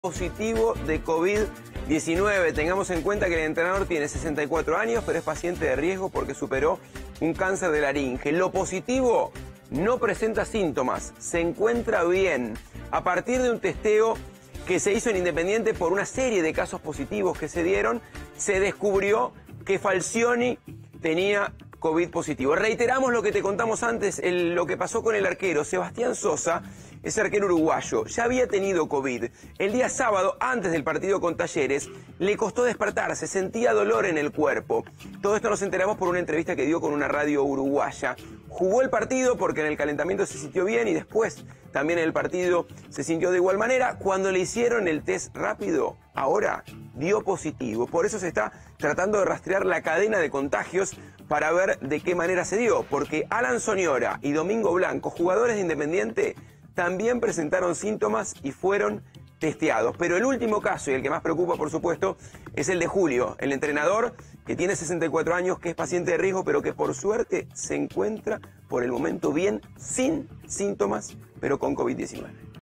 Falcioni tiene coronavirus
El DT de Independiente es paciente de riesgo